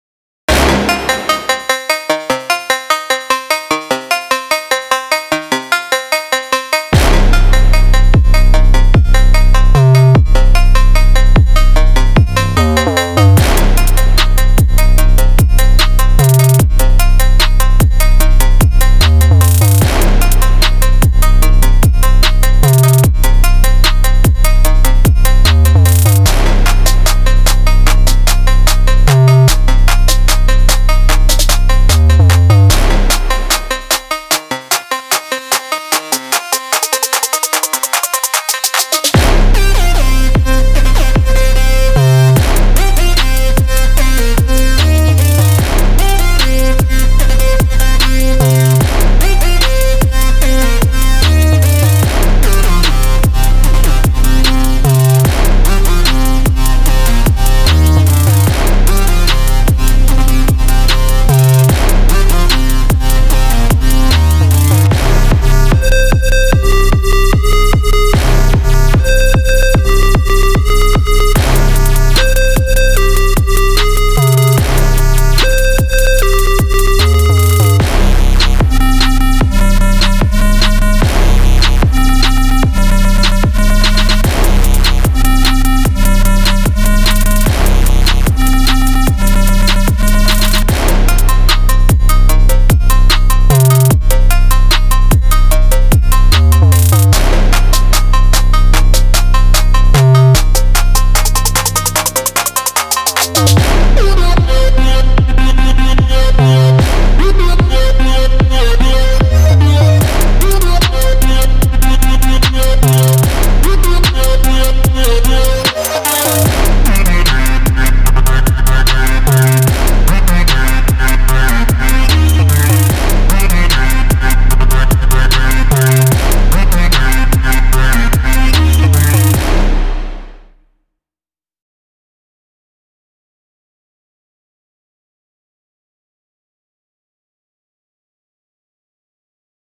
TRAP jam on the Novation Circuit
An EDM / hip hop trap jam on the Novation Circuit, using my free drum pack and synth patches from BoBeats and Tom Cosm.